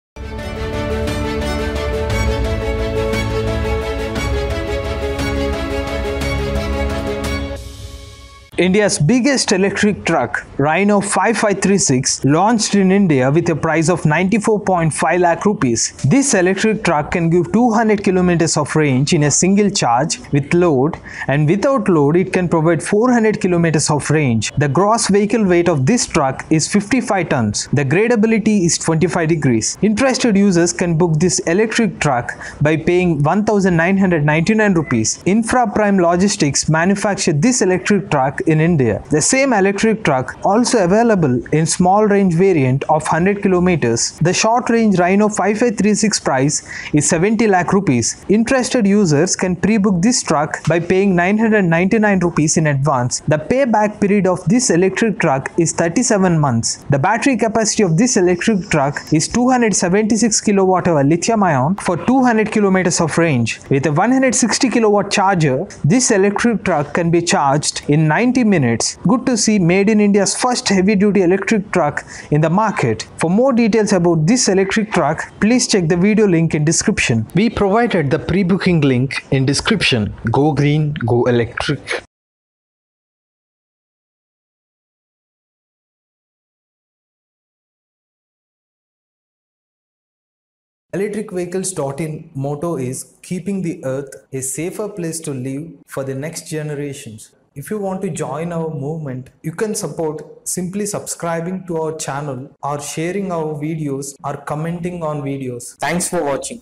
India's Biggest Electric Truck Launched - Rhino 5536
India's Biggest Heavy duty electric truck Rhino 5536 Launched in the market.
Iptl tech Electric truck specs
Range - 200 km
Battery - 276 kWh

eScooter Spare Parts
"Hero Electric Optima Bike
Hero Electric Photon Bike